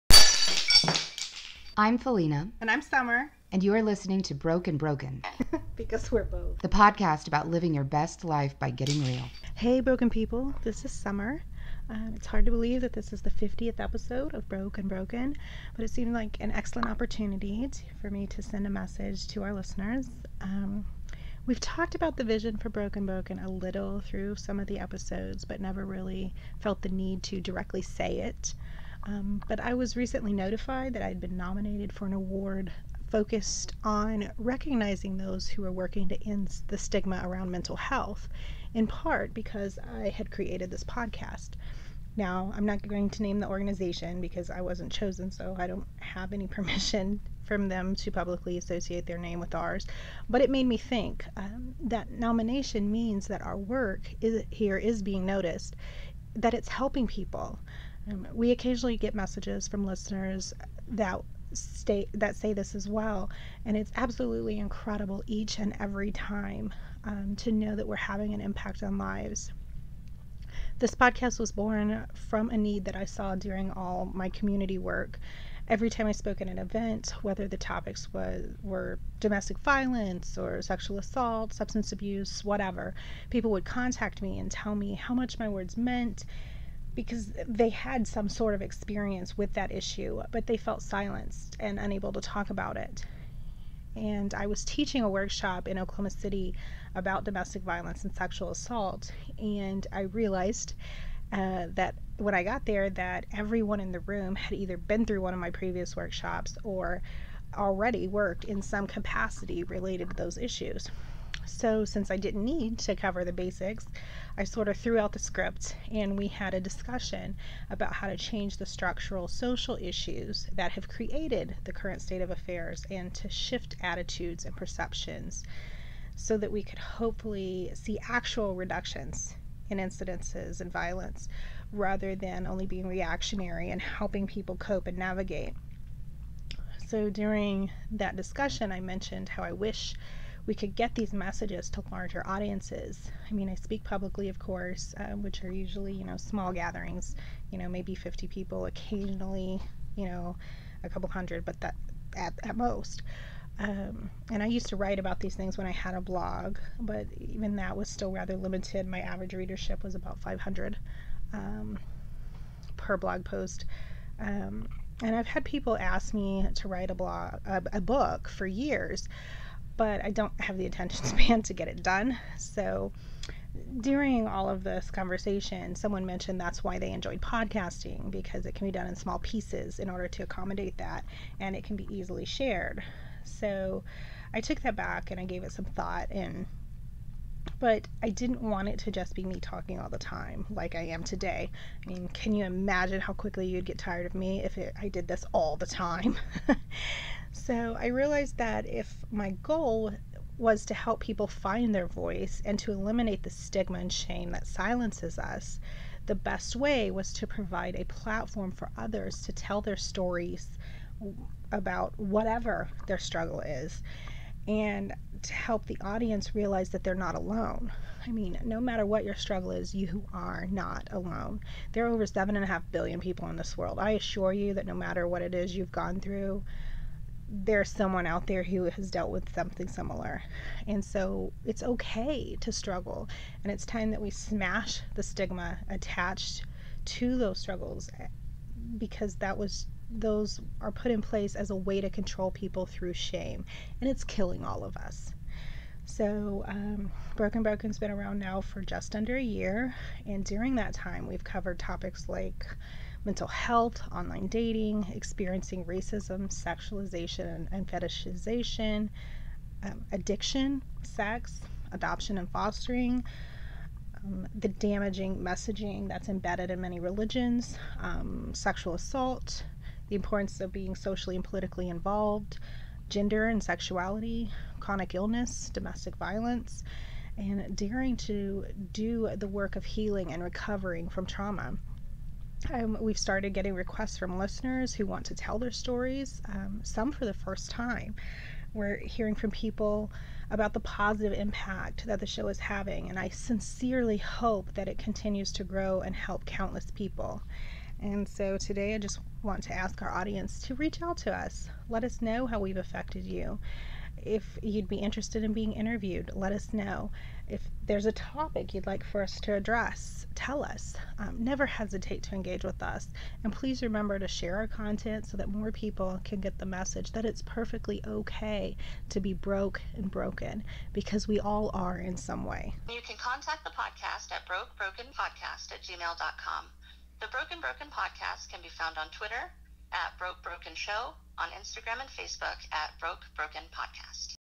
Episode 50: Broke & Broken
Episode 50 seems like the perfect chance for a quick message about how Broke & Broken was created and how we hope to continue to serve the community. We hope out listeners continue to engage with us, and help spread our content so that it can have further impact. Together we can eliminate stigma and the shame that silences us, so that we can live our best lives and improve our mental health.